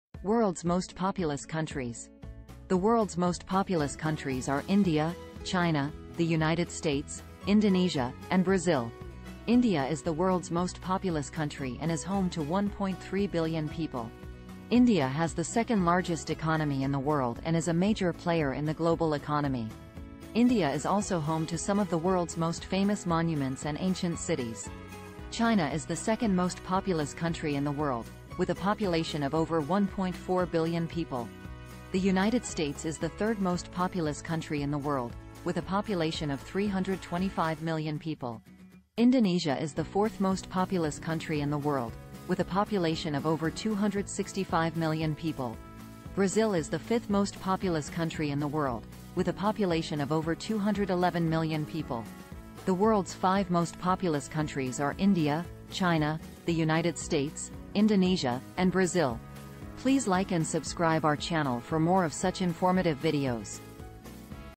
World’s Most Populous Countries
The world’s most populous countries are India, China, the United States, Indonesia, and Brazil. These five countries have a combined population of nearly 3 billion people and make up more than one-third of the world’s total population. Each of these countries has its own unique history, culture, and challenges

The world's population is incredibly diverse, and it's dominated by a few countries with staggering numbers. The five most populous countries are:

China: With over 1.4 billion people, China is the most populous nation on Earth.

India: India is a close second, with more than 1.3 billion inhabitants, and its population is still growing rapidly.

United States: The USA has a population of around 330 million people and is one of the most diverse countries globally.

Indonesia: Home to over 270 million people, Indonesia is the fourth most populous country.

Pakistan: With approximately 220 million people, Pakistan rounds out the top five most populous nations.

These countries not only boast large populations but also play significant roles in global economics, politics, and culture due to their sheer size and influence.